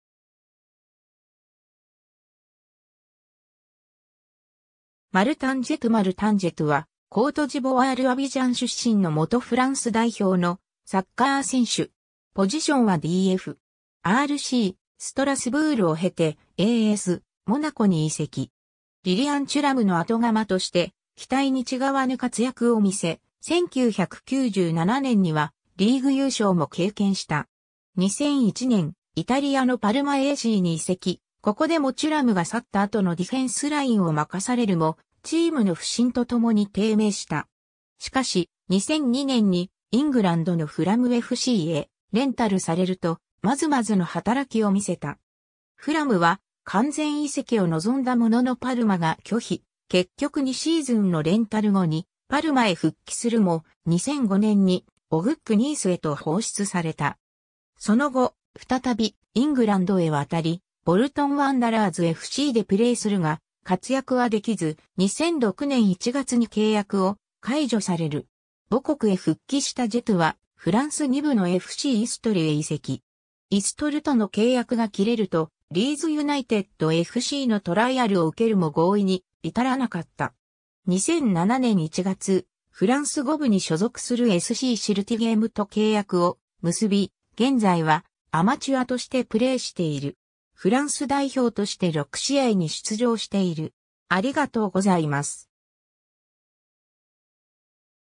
マルタン・ジェトゥ
ボルトン・ワンダラーズFCの選手
マルタン・ジェトゥ マルタン・ジェトゥ（Martin Djetou, 1974年12月15日 - ）は、コートジボワール・アビジャン出身の元フランス代表のサッカー選手。
 RCストラスブールを経て、ASモナコに移籍。
リリアン・テュラムの後釜として期待に違わぬ活躍を見せ、1997年にはリーグ優勝も経験した。
2001年、イタリアのパルマACに移籍、ここでもテュラムが去った後のディフェンスラインを任されるも、チームの不振とともに低迷した。
しかし、2002年にイングランドのフラムFCへレンタルされると、まずまずの働きを見せた。
フラムは完全移籍を望んだもののパルマが拒否、結局2シーズンのレンタル後にパルマへ復帰するも、2005年にOGCニースへと放出された。
 その後、再びイングランドへ渡り、ボルトン・ワンダラーズFCでプレーするが活躍はできず、2006年1月に契約を解除される。
母国へ復帰したジェトゥはフランス2部のFCイストルへ移籍。
イストルとの契約が切れると、リーズ・ユナイテッドFCのトライアルを受けるも合意に至らなかった。
2007年1月、フランス5部に所属するSCシルティゲイムと契約を結び、現在はアマチュアとしてプレーしている。
 フランス代表として6試合に出場している。